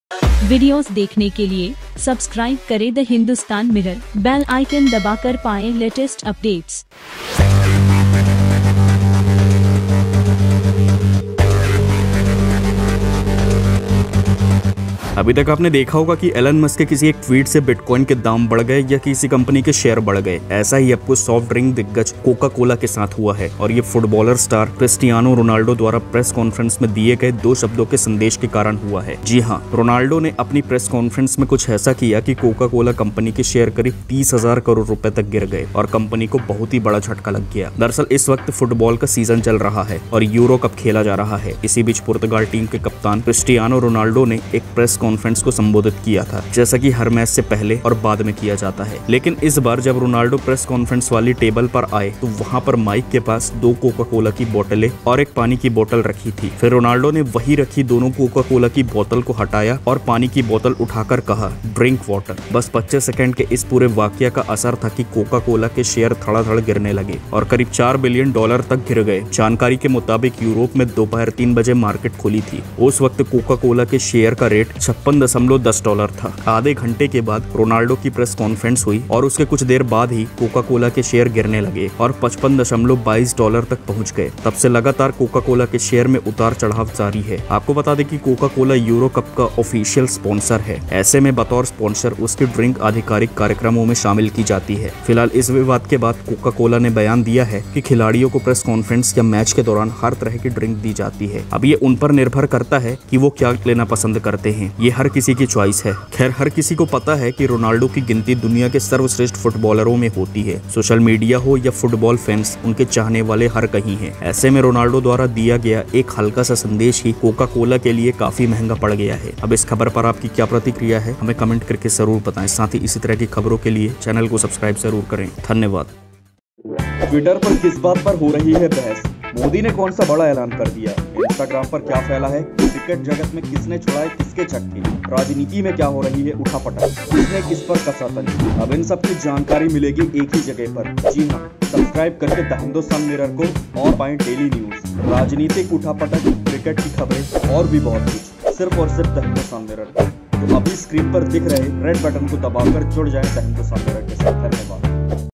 Coca Cola Lost $4 Billion After Ronaldo's Statement | Cristiano Ronaldo On Coca Cola |Coca Cola News
Video Title :-

Video Description :-
Cristiano Ronaldo’s removal of two Coca-Cola bottles during a press conference at the European Championship has coincided with a $4bn fall in the share price of the drinks company. For full information watch this video.